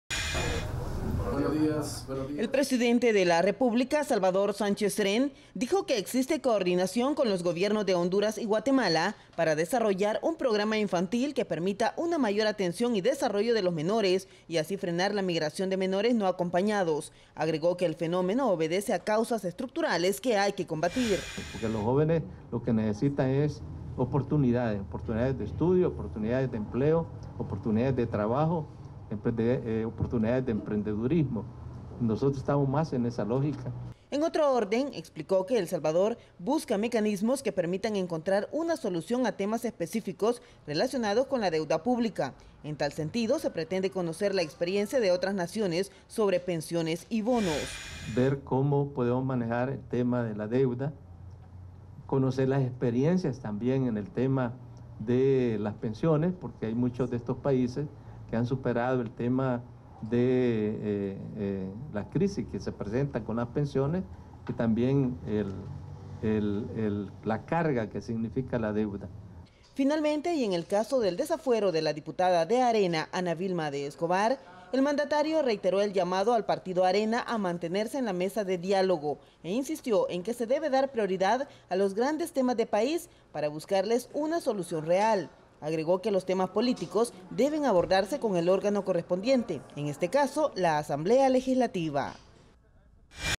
Gentevé Noticias - Presidente Cerén trata temas de importancia para la nación
El Presidente Cerén también se refirió a varios temas de interés nacional entre ellos, la solución al drama humano que enfrentan varios niños salvadoreños retenidos en un albergue en Estados Unidos